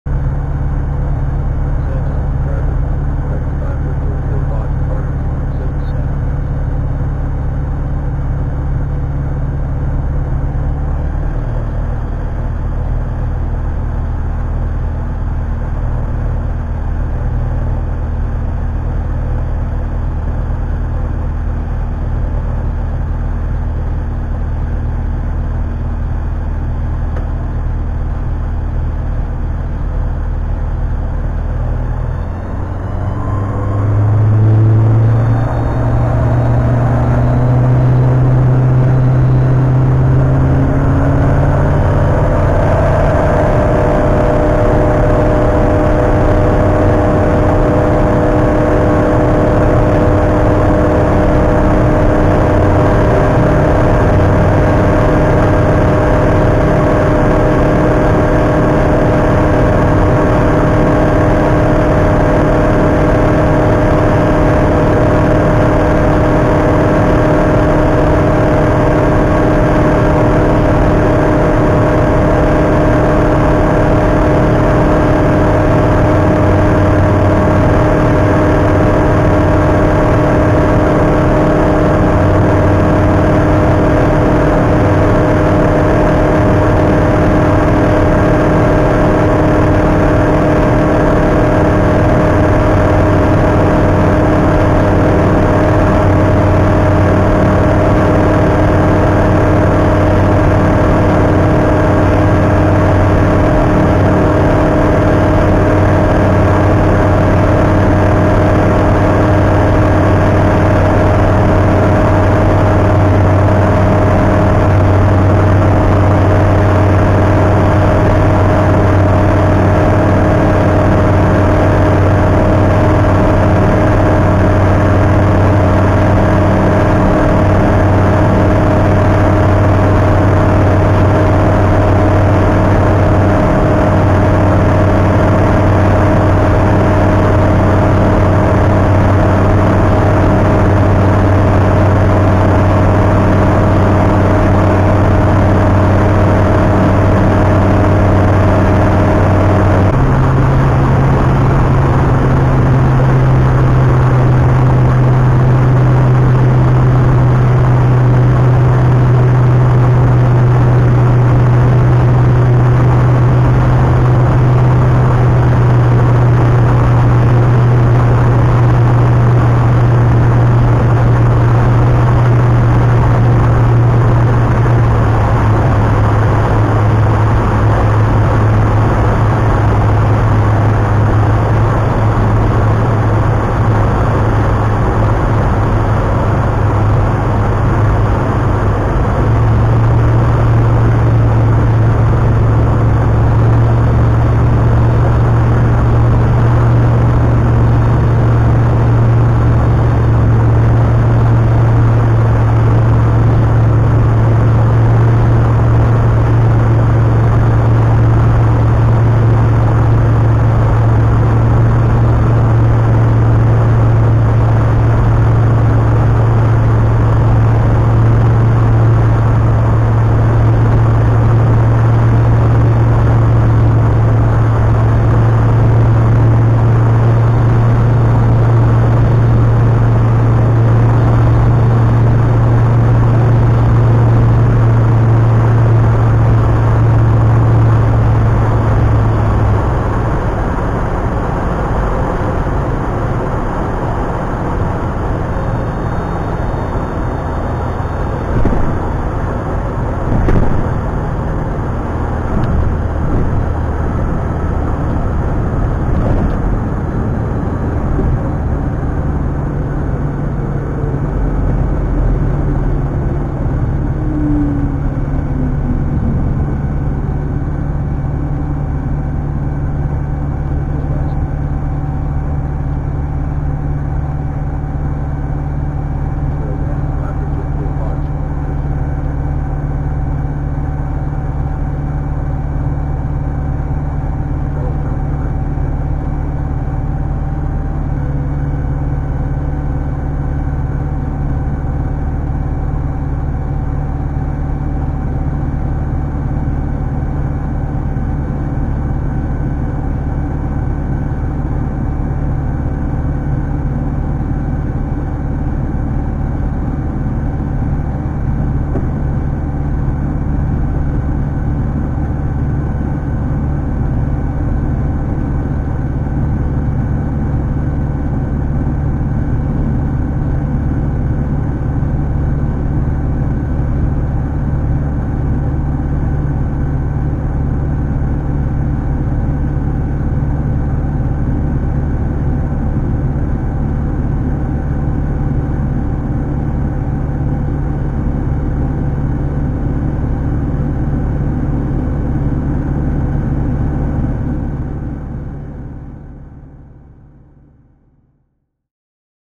Tecnam Astore Landing at MOB
Nice view of Garmin SafeTaxi automatically zooming in after landing. I wish Foreflight would do this!